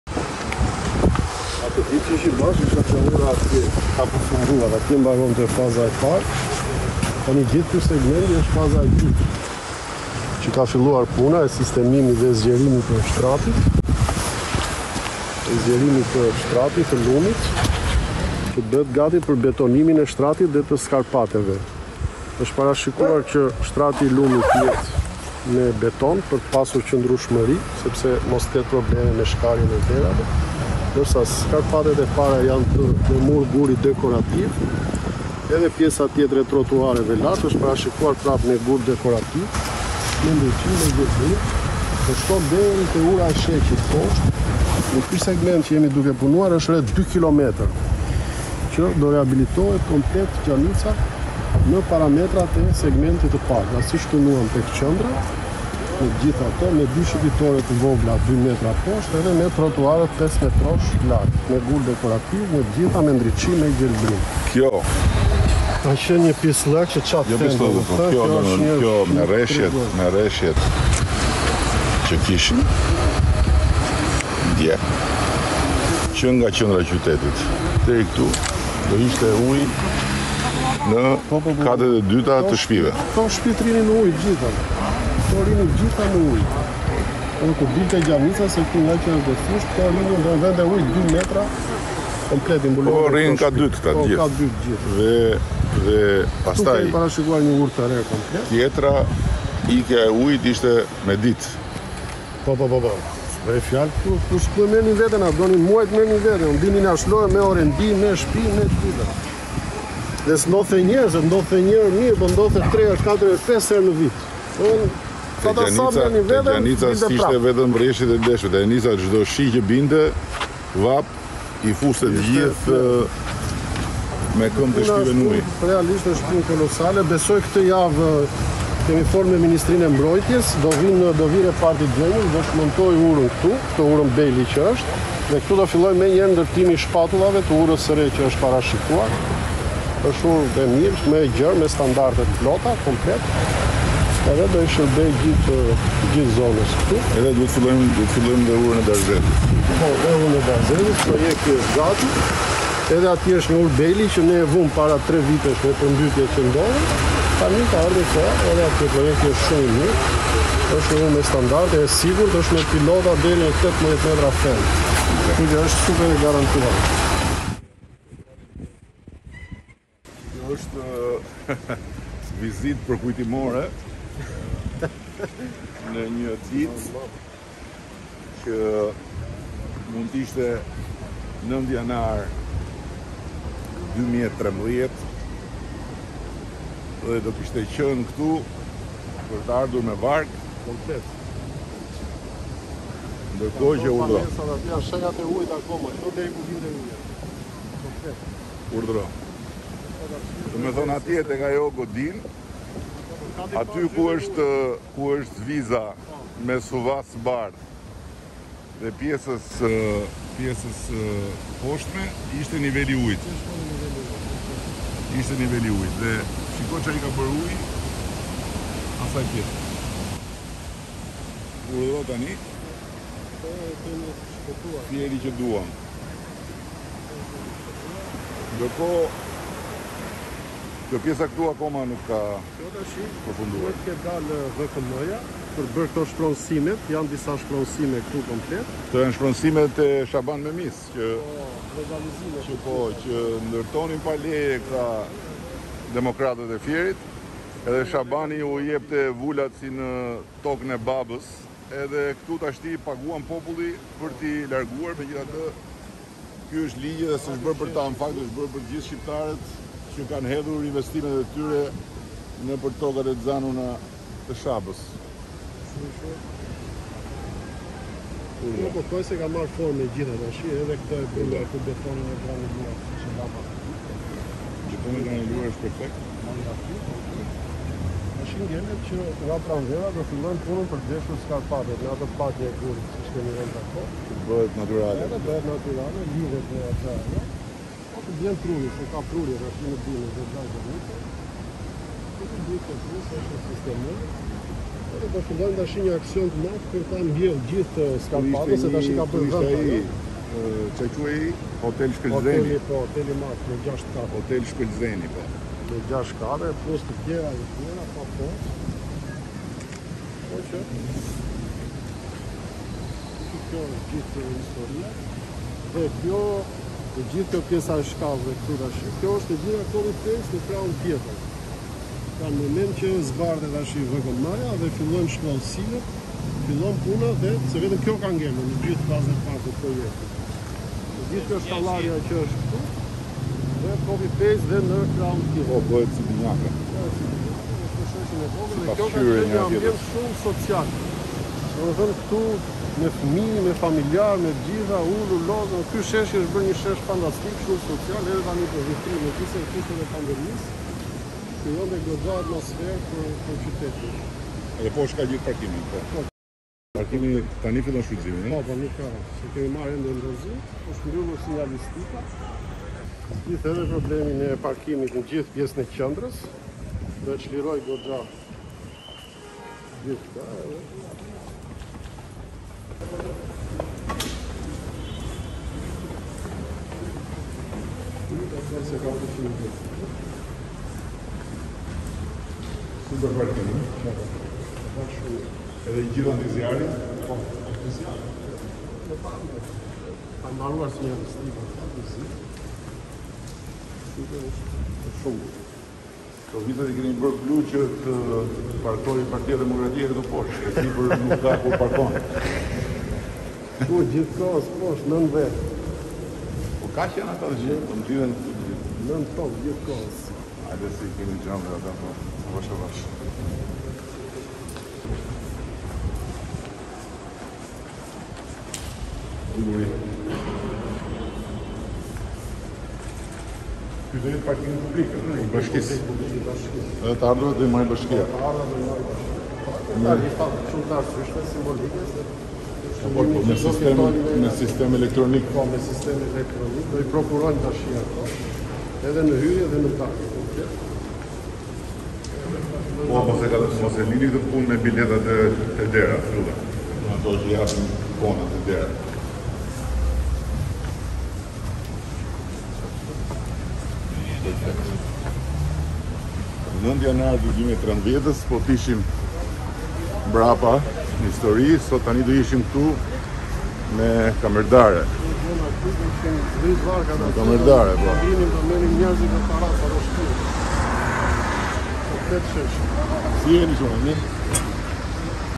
Kryeministri Edi Rama - "Vizitë përkujtimore" përgjatë lumit Gjanica
' Fier - "Vizitë përkujtimore" përgjatë lumit Gjanica, që po të ishim sot në 9 janarin e vitit 2013 do ta kishte kthyer zemrën e Fierit në një kënetë pisllëku, siç ndodhte jo vetëm pas rreshjesh aq të dendura sa ditët e fundit, po pas çdo shiu vere, kur Gjanica e bllokuar nga plehu i mbledhur në vite mes dy varganëve të ndërtimeve pa leje në anët e saj, transformohej në një monstër uji që hynte në çdo shtëpi e biznes, duke bërë dëme shumëmilionëshe🤮
Ky është dallimi mes vizionit dhe verbërisë, dashurisë për vendin e njerëzit dhe llafollogjisë boshe mbi plehun e përtacisë👊